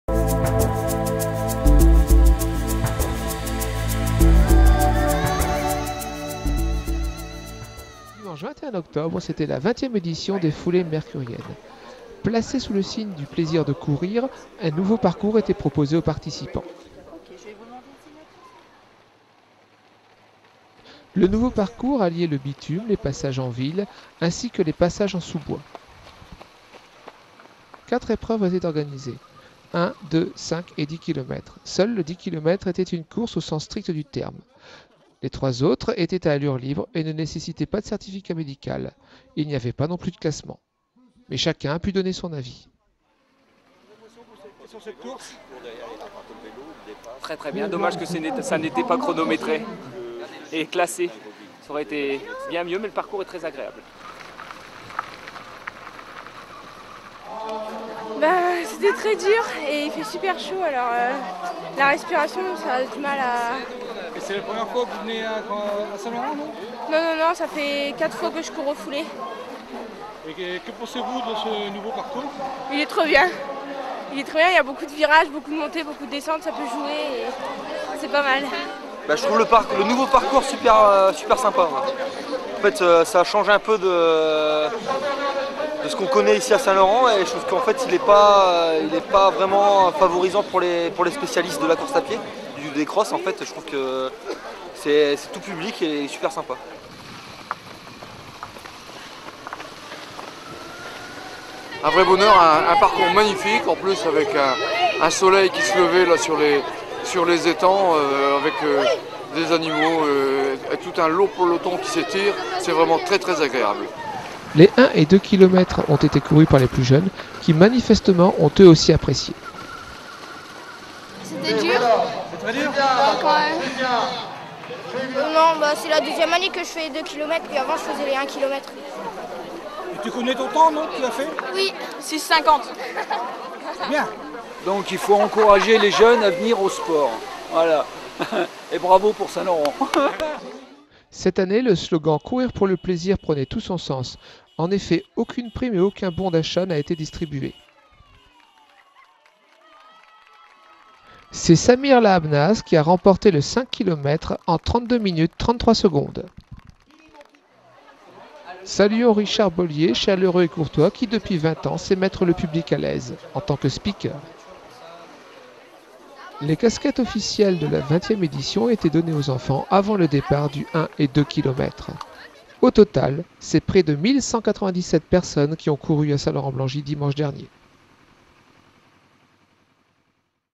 FOULEES IMMERCURIENNES : COURIR POUR LE PLAISIR
Vingt ans, et pas une ride ! Dimanche 21 septembre, les Foulées Immercuriennes ont rassemblé de nombreux sportifs pour une édition inédite.Si les distances de 1, 2, 5 et 10 kilomètres n'ont pas changé, leur itinéraire a quelque peu varié. Le nouveau parcours, qui traversait notamment les parcs immercuriens, a séduit les coureurs. Mis à part les 10 kilomètres qui gardent leur label départemental, les trois autres circuits ont, cette année, été courus à allure libre.Le dévouement des nombreux bénévoles immercuriens et le beau temps au rendez-vous ont, eux aussi, contribué à la réussite de ce grand événement sportif.